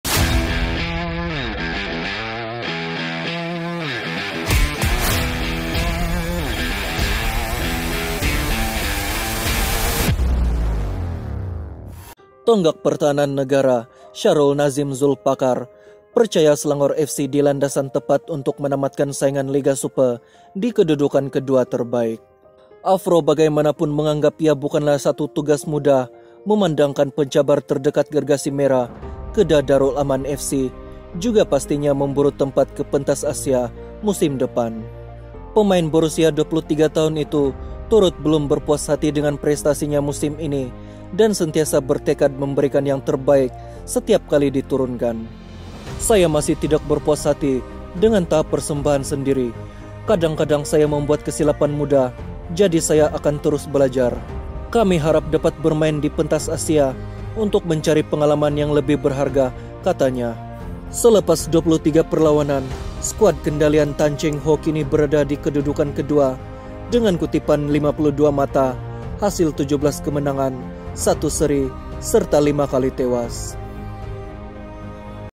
Sharul Nazeem Yakin Selangor Kekal Naib Juara Liga Super.
Tonggak pertahanan negara, Sharul Nazeem Zulpakar percaya Selangor FC di landasan tepat untuk menamatkan saingan Liga Super 2023 di kedudukan kedua terbaik.